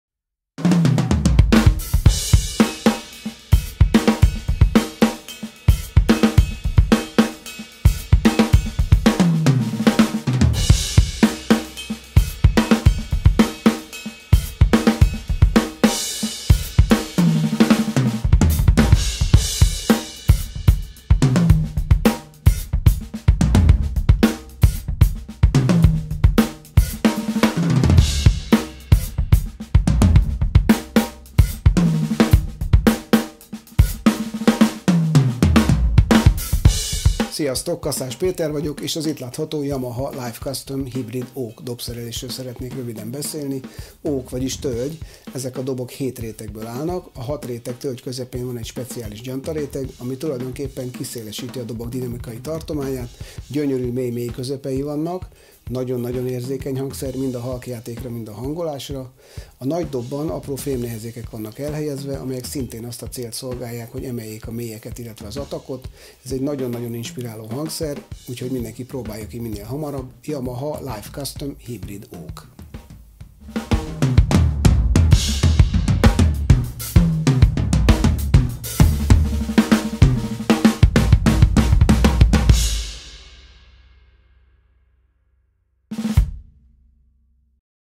Yamaha Live Custom Hybrid Oak | Péter Kaszás​ | Drums | Yamaha Music | Magyar
Kaszás Péter hazánkban és nemzetközileg is elismert többszörös díjnyertes dobos, zeneszerző és énekes.

Az elmúlt években számos magyar zenekarban megfordult, éveken keresztül turnézott a világhírű gitárművésszel Al Di Meolával.

Ebben a rövid videóban bemutatja a Yamaha Live Custom Hybrid Oak dobszerelést.

Péter Kaszás is a domestically and internationally renowned multi-award winner drummer, composer and singer.

He has played in various Hungarian bands in the recent years, was touring for years with the world-famous guitar artist Al Di Meola.

In this short video he is presenting the Yamaha Live Custom Hybrid Oak Drum Set.